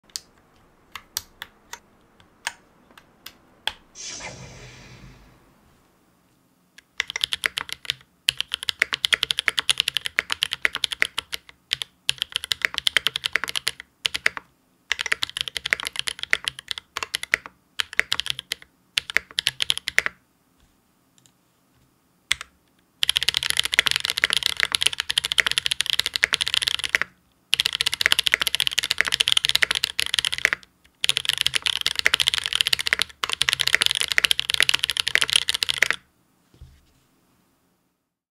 Holy Panda on SPACE65
Keyboard: SPACE65 R1
Plate: Carbonfiber
Switches: Drop Invyr Holy Panda (Krytox205g0, no film)
Stabilizers: Everglide V2 Screw-in (Krytox XHT-BDZ)
Keycaps: GMK Oblivion V1

🐱 Neko Atsume Figure
📷 Sony a7C Camera
📷 Sony SEL35F18F 35mm F1.8 Lens
🎙 Shure MV7